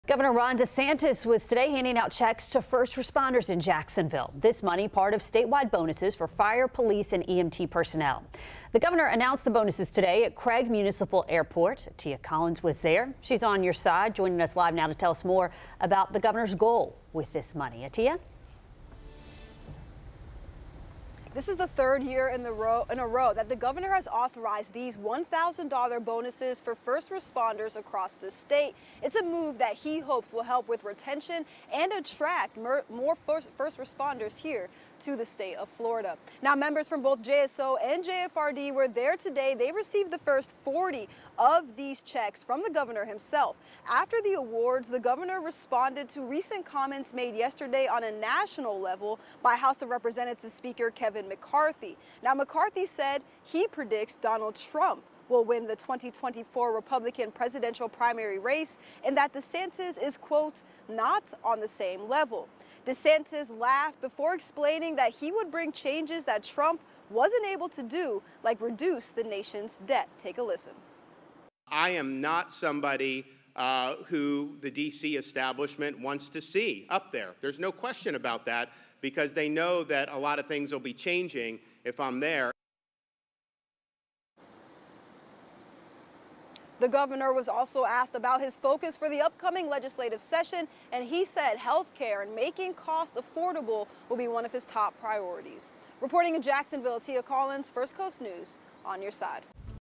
Gov. DeSantis announces $1,000 checks for first responders
It's the third year in a row the governor has announced bonuses for first responders. JSO and JFRD members received the first 40 of those checks in-person Monday.